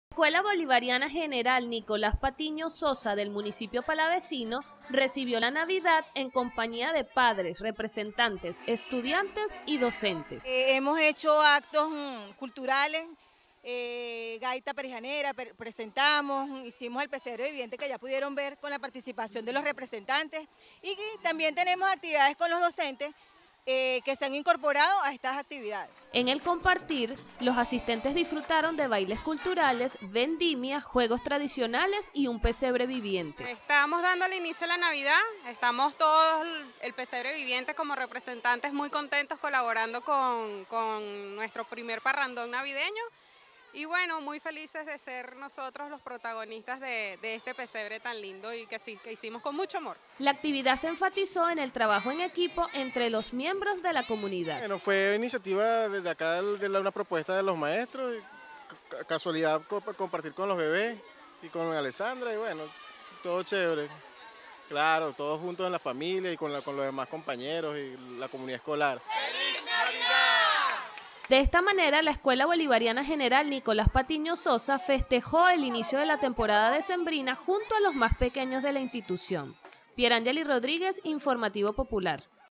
La Escuela General Nicolás Patiño Sosa Recibió La Navidad
En el municipio Palavecino del estado Lara, la Escuela Bolivariana Gral. Nicolás Patiño Sosa dió inicio a la navidad junto a la comunidad en la actividad se resaltaron los valores culturales a través de juegos y bailes.